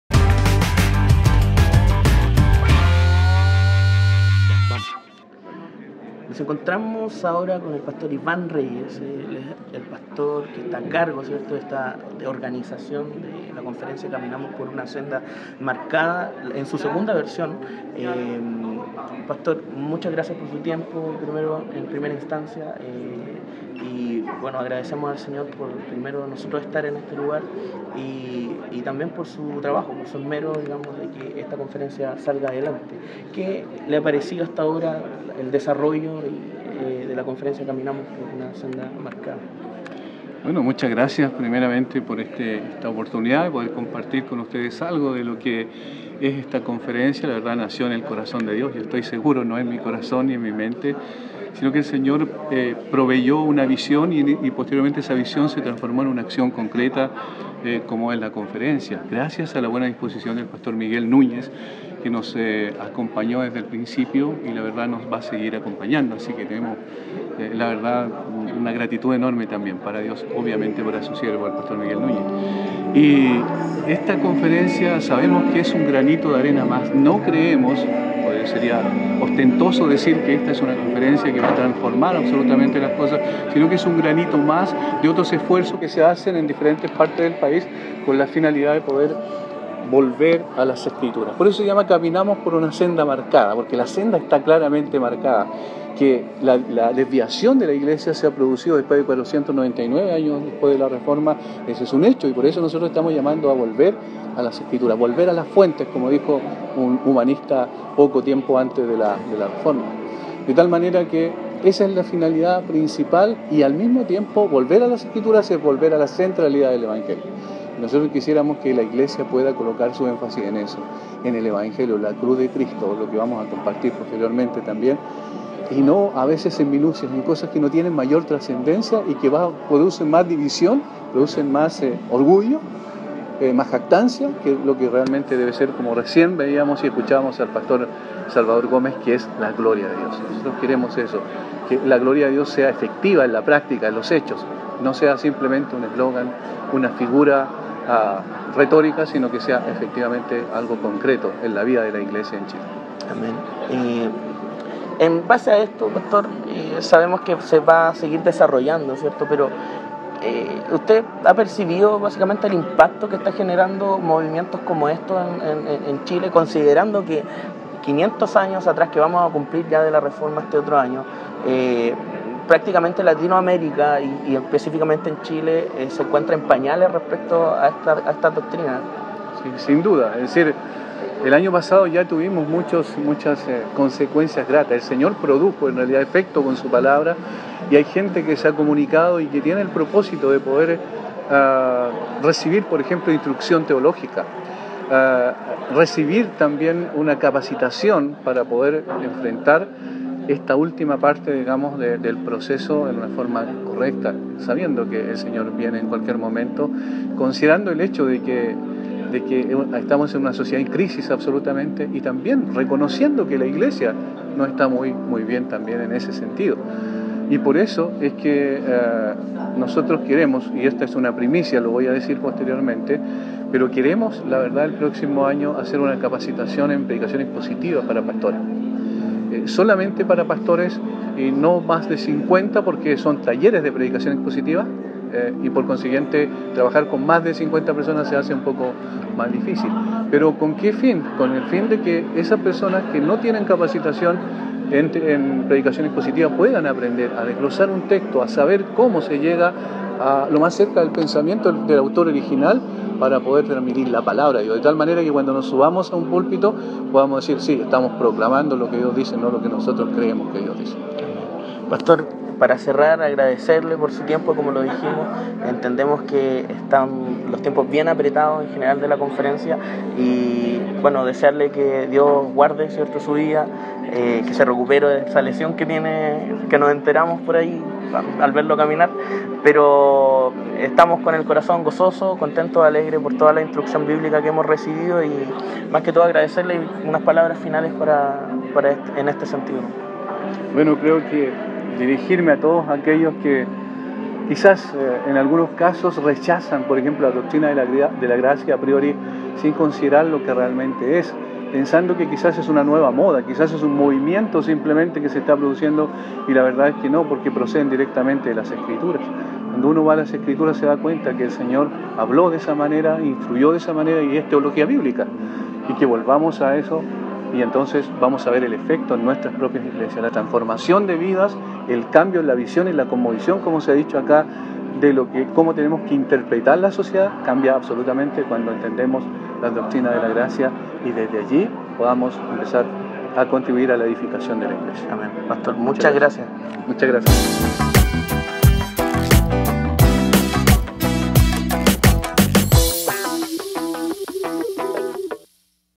Entrevista Pastor Iván Reyes (Caminamos por una senda marcada )
En el marco de la Conferencia "Caminamos por una senda marcada" tuvimos la oportunidad de conversar con el Pastor Iván Reyes, Pastor anfitrión de la misma conferencia y que en esta casión estuvo participando como uno de los expositores Bíblicos. Sebastián nos cuenta más en esta entrevista que sabemos será de edificación para ti.